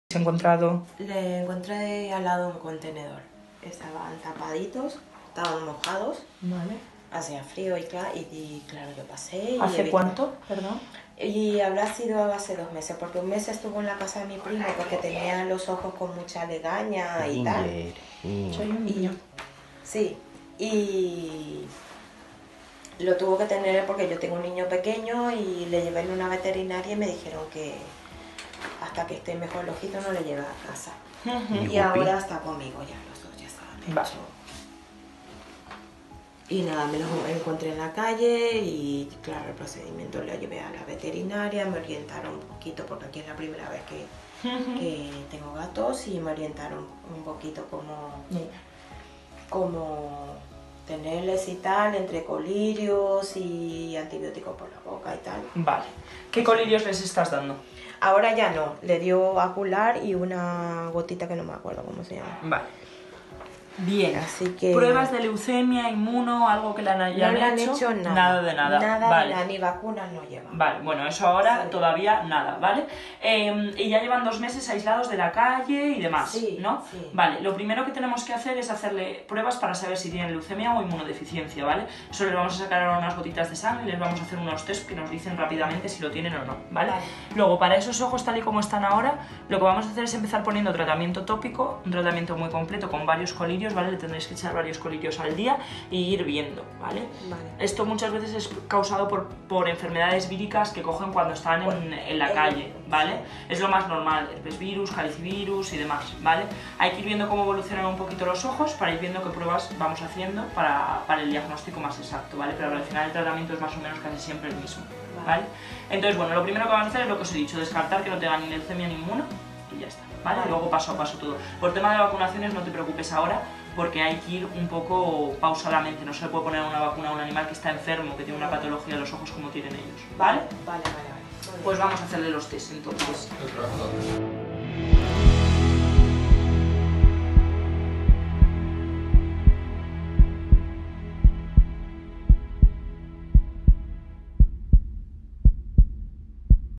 Ginger y Guppy, dos gatitos tirados a un contenedor de basura con alteraciones oculares graves
Ginger y Guppy fueron rescatados hace dos meses de un cubo de basura siendo unos cachorros de apenas unos días. Siempre tuvieron los ojitos mal y su humilde familia rescatadora ha hecho todo lo que estaba en su mano para poder ayudarles, pero el bolsillo no les da para más.
Cuando conocimos su caso, no tuvimos dudas en ayudarles. Han acudido a nuestro centro, donde les hemos revisado y puesto tratamiento.
También hemos aprovechado para realizarles tests de inmunodeficiencia y leucemia, siendo negativos a ambas los dos gatitos.
En cuanto mejoren, también empezaremos con su identificación, vacunas y castración.